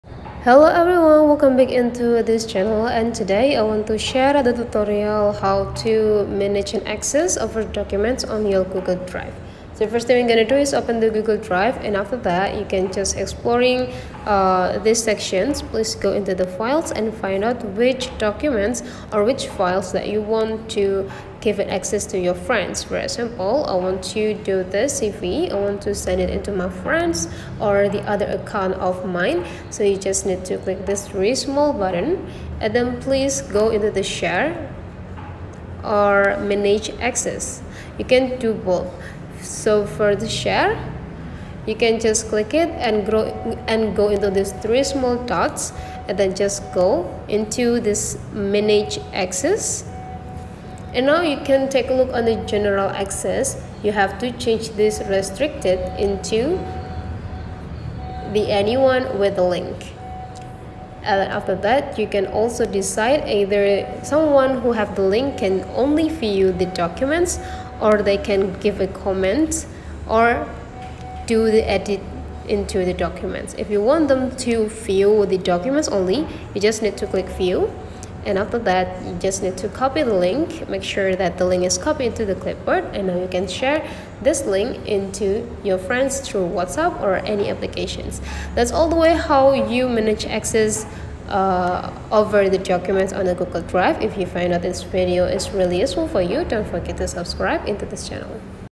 How to Give Someone an Access of a Files in Google Drive? Share Your Files by Google Drive!
Share the documents without download the file into your phone! You can give an access into your friends about the documents so that your friends able to see your documents too. Here is how to manage access on google drive by iphone.